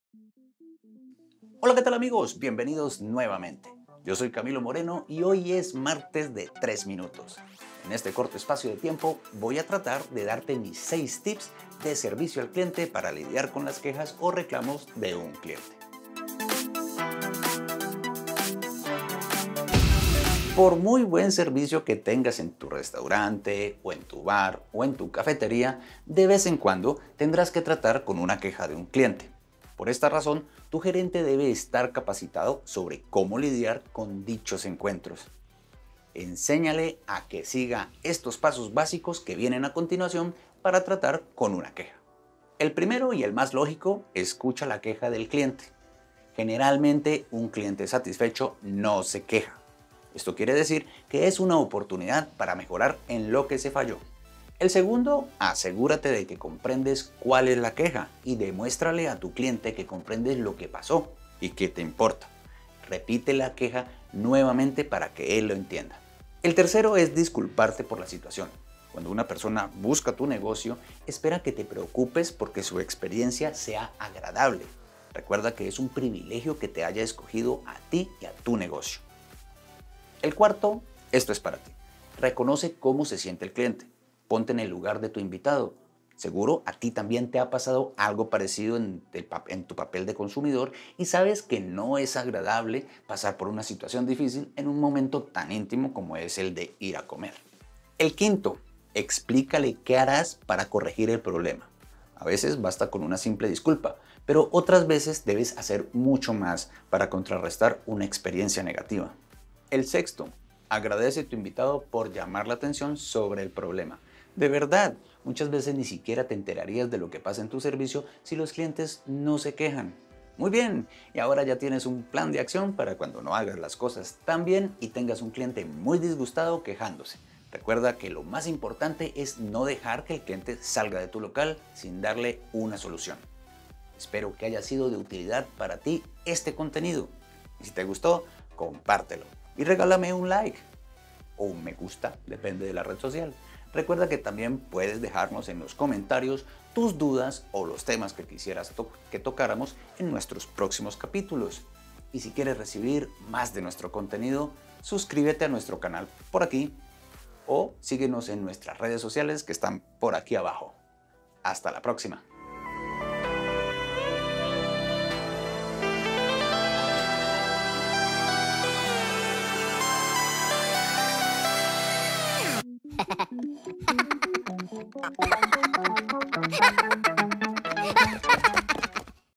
Cómo lidiar con las quejas o reclamos de los clientes- Marketing Gastronómico
Hola Amigos, hoy es martes de tres minutos y esta ves vamos a darte 6 tips para que te puedas guiar y saber que hacer cuando No hagas las cosas "TAN BIÉN" y tus clientes tengan un reclamo de cualquier índole, ya sea de servicio o sobre el producto... recuerda que una de las cosas más importantes en el servicio al cliente, es que te puedas comunicar con ellos, que los escuches y que se enteren de que te interesa lo que ellos piensan y/o desean de tu negocio.

Estrategia Digital
Estrategias Gastronomicas
Régimen Operativo.
Capacitación del personal en servicio al cliente.